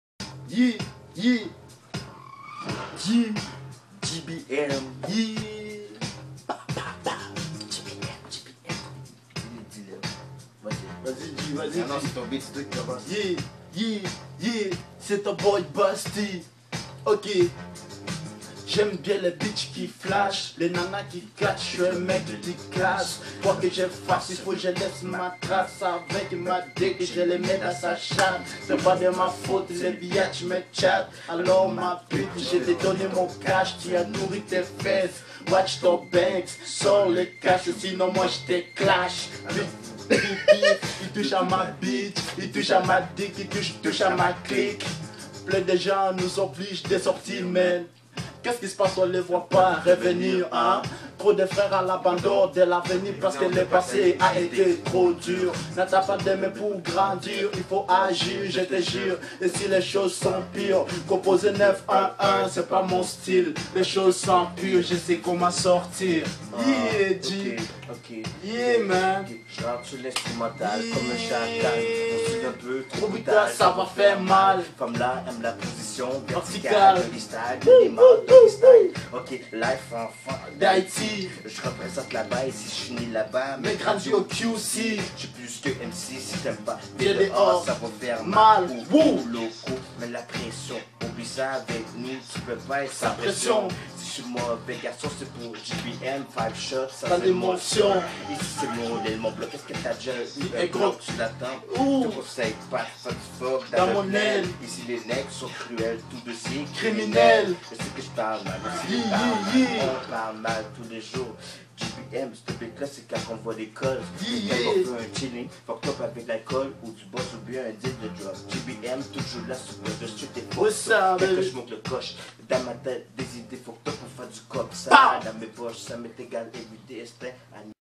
GiGa-Freestyles 3 - Basty & Locko from GBM - Le DeN Studio
freestyles GBM au DeN Studio filmé par Mounga (dis moi si jler mal ecrie g ^^)